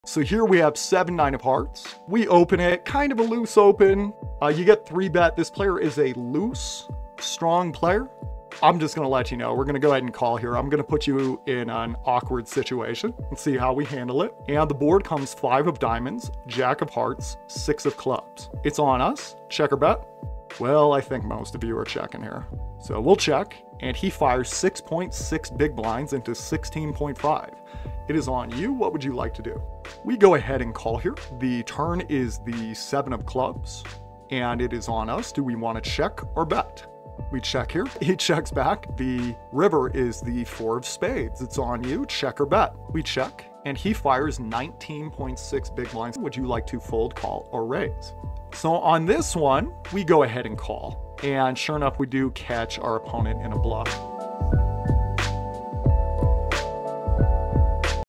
New JAKACoaching Coach Teaches Exploits
Welcome our latest JAKAcoaching coach Alex Fitzgerald @pokerheadrush!

He's a long-time pro, coach, and well-known author of some fantastic poker books. Let's talk about this hand he broke down in a recent lesson he did with our JAKAcoaching students.

This is a great example of playing a strong, loose player out of position.

We check call the flop here and the turn goes check check

We check the river and our opponent bets 19.6 BB into 29.7 BBs. Some players may have been tempted to bluff on the river, but our hand is too high up on our range to turn a hand that already has good showdown value into a bluff.

If we had a hand like A5s or AQs with less show-down value, it would make more sense to consider turning those into bluffs. When you have a hand that has enough showdown value to win the pot, but not quite good enough to get called by worse, you want to use it as bluff catchers. In this instance, our opponent was loose and aggressive and we blocked 98s and 87s which made it less likely they had an 8, so Alex decided to make the hero call and dragged in a nice pot!